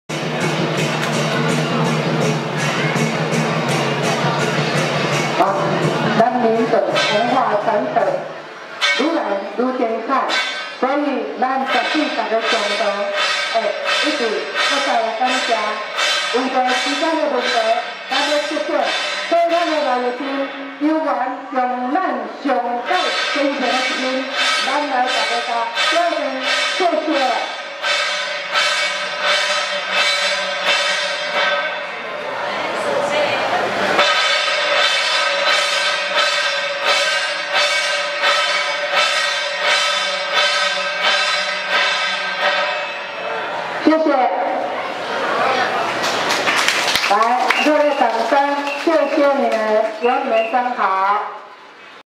丙申年高雄草衙朝陽寺觀音佛祖平安遶境 轎前鑼陣 入廟表演 [4K-UHD]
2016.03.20 天運歲次丙申年 農曆二月十二日 高雄前鎮舊草衙朝陽寺慶祝建寺342週年觀音佛祖平安遶境大典